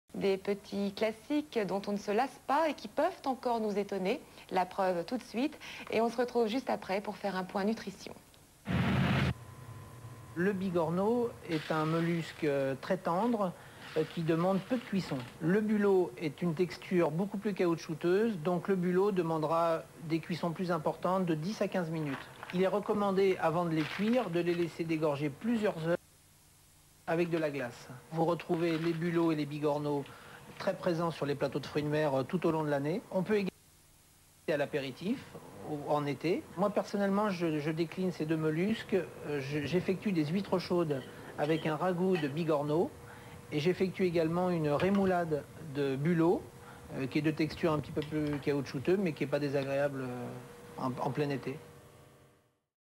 Petit conseil de preparation des bigorneaux et bulots
Quelques idées de preparation des bigorneaux et bulots par Gérard Sallé.